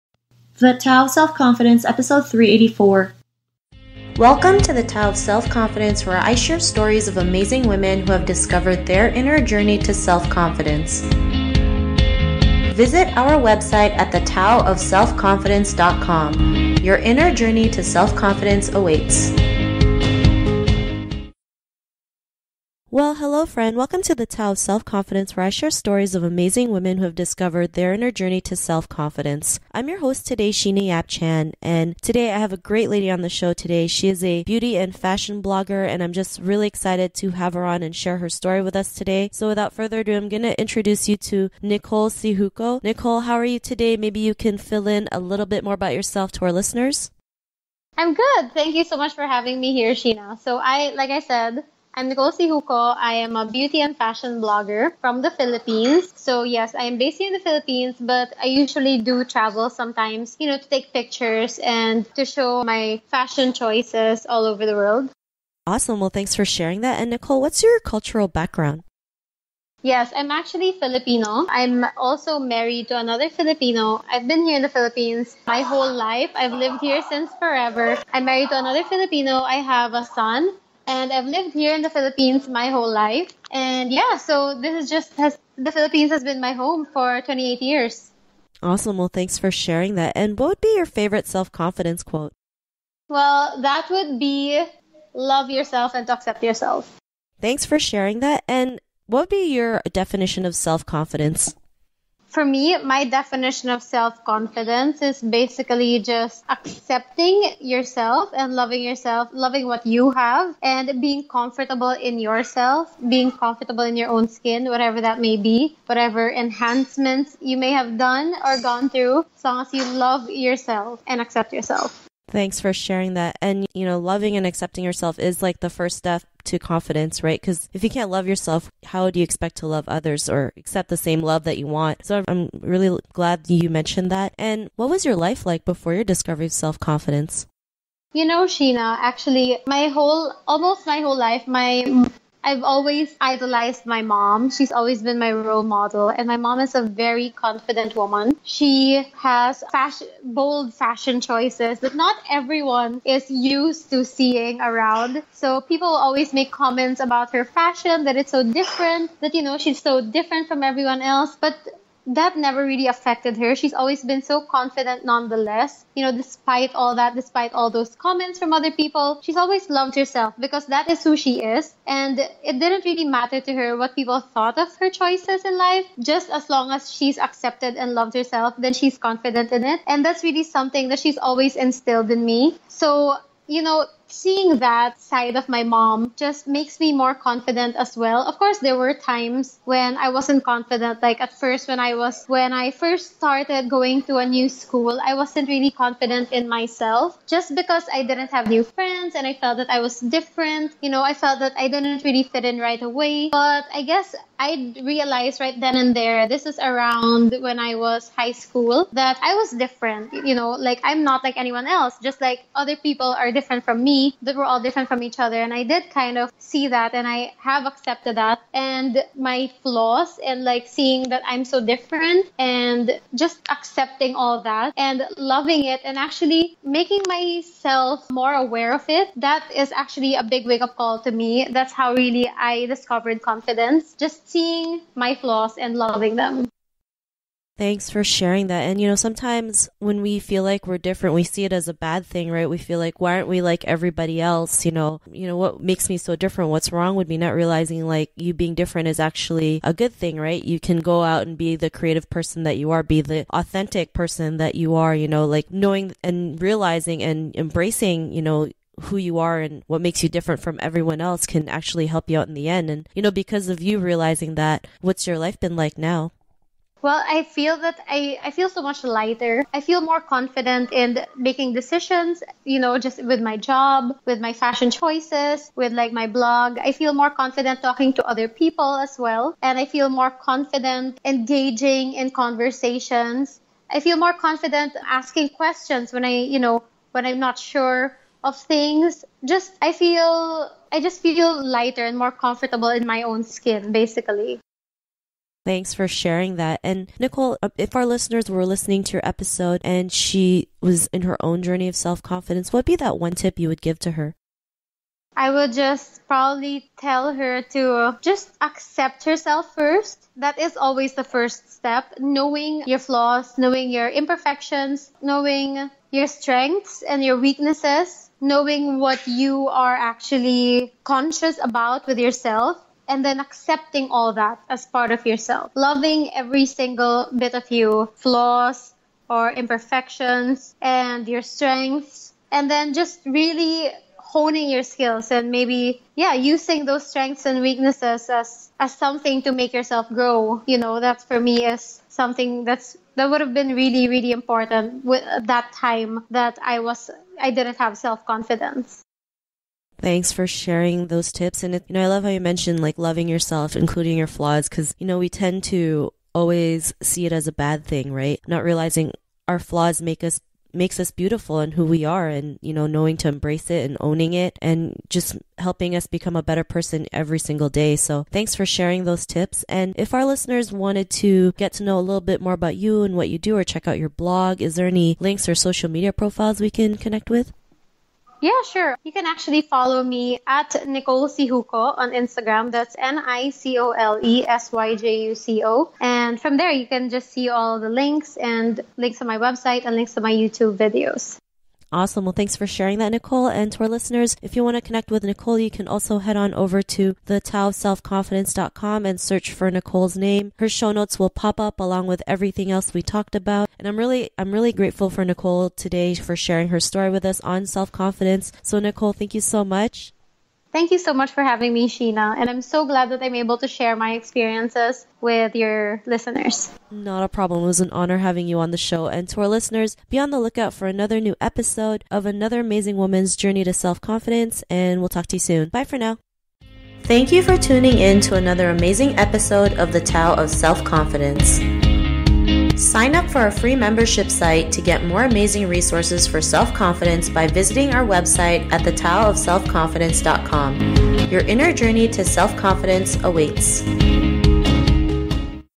384: Being Different Is Good With Nicole Syjuco
Nicole Syjuco is a Makeup Blogger and Youtube Beauty Guru from the Philippines.  Beauty is her passion.  She loves everything beauty and makeup related. Nicole runs a beauty blog which she shares with you her make up techniques and tricks, her make up tutorials, her favorite products, and reviews/swatches of makeup products.

Nicole used to lack self confidence because she was different from everyone else.  When she realized that being different is a good thing, that is when her confidence grew and became her true self in the process.

Check out thetaoofselfconfidence.com for show notes of Nicole's episode, Nicole's website, resources, gifts and so much more.Want to manifest like a queen?